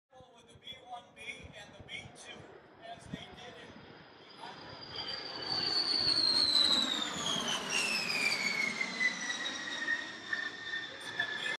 B-52 Flyby Kaneohe Bay
Video by Tech. Sgt. Amanda Dick Pacific Air Forces Public Affairs

A U.S. Air Force B-52 Stratofortress deployed to Andersen Air Force Base, Guam, performs a flyby during the Kaneohe Bay Air Show, Hawaii, Oct. 17, 2015. The aircraft completed the flyby while on a routine flying missing, showcasing its capability to fly from Guam to Hawaii and back without stopping. (U.S. Air Force video by Tech. Sgt. Amanda Dick)/Released